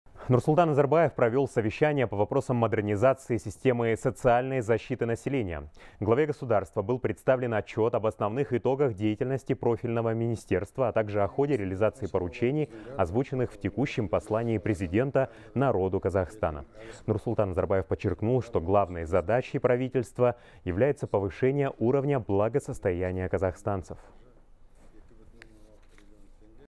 Совещание по вопросам модернизации системы соцзащиты населения прошло в Акорде
Совещание по вопросам модернизации системы социальной защиты населения под председательством Главы государства прошло в Акорде. В ходе совещания Главе государства был представлен отчет  об основных итогах деятельности  Министерства труда и социальной защиты населения, а также о ходе реализации поручений, озвученных в текущем Послании Президента народу Казахстана.